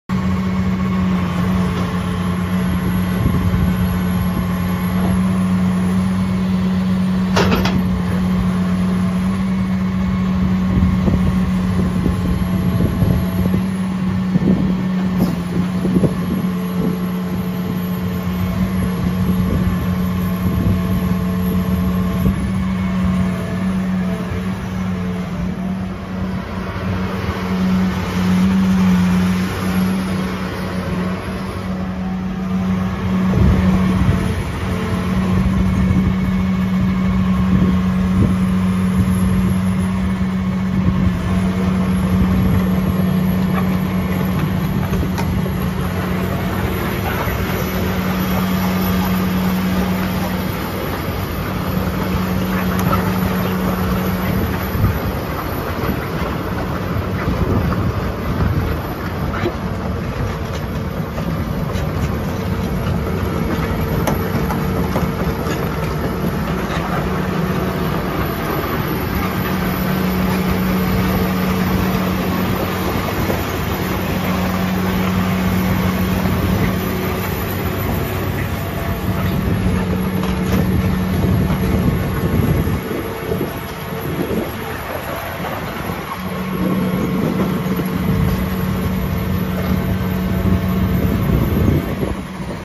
2005 DEERE 200C LC For Sale
Fleet Maintained, Cab, 6543 Hours, Heat & Air, Radio, 2 Speed, 140 HP John Deere Diesel, 48” Tooth Bucket, Power Coupler, 32” Steel Tracks, 22' Max Digging Depth, 44754 LBS Operating Weight, Located at McGrew Equipment Company (Seven Valleys PA)

 [2005 JOHN DEERE 200C LC EXCAVATOR]